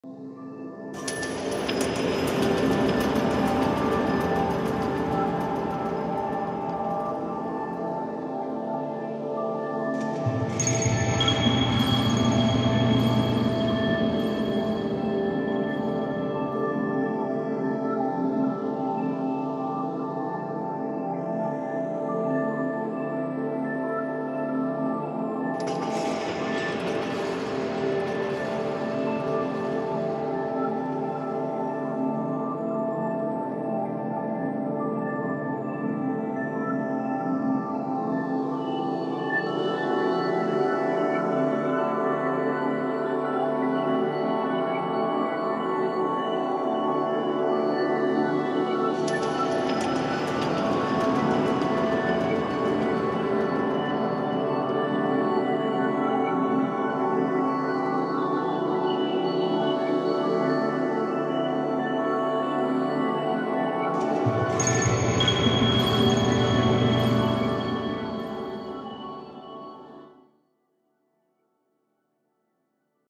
Polar Candle in Yokohama
This work is a trick picture installation that uses a mirror.